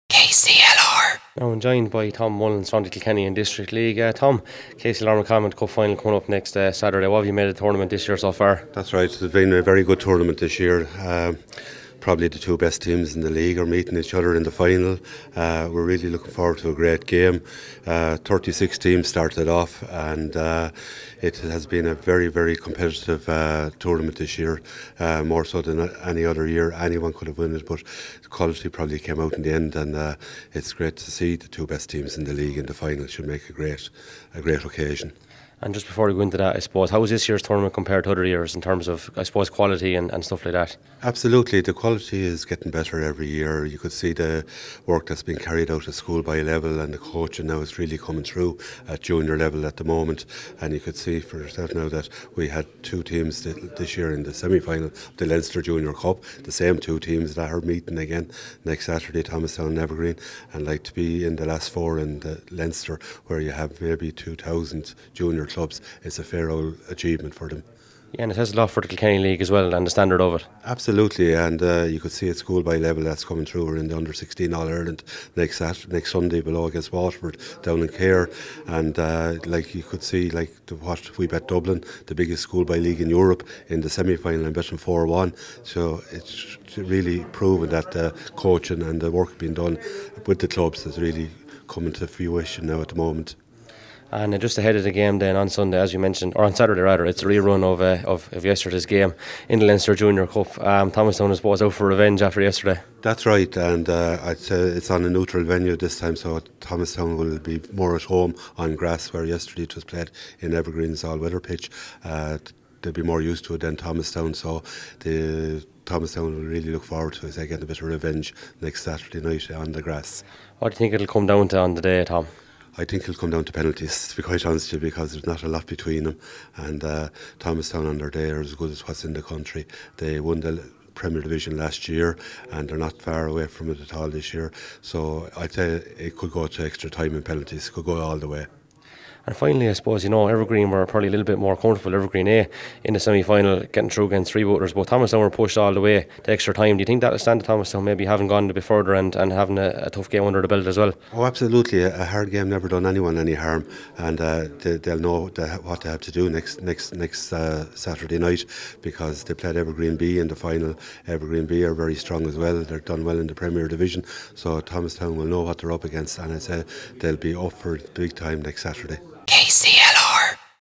KCLR McCalmont Cup Preview: Tom Mullins Interview (KDL)
Kilkenny & District League's Tom Mullins talks to Kevin Regan about the 2016 KCLR sponsored McCalmont Cup Final between Thomastown and Evergreen.

What will the outcome be for the two best sides in the league this year?

Tune in at 96-96.9FM in Carlow and Kilkenny or listen

KCLR - The Heart of Carlow Kilkenny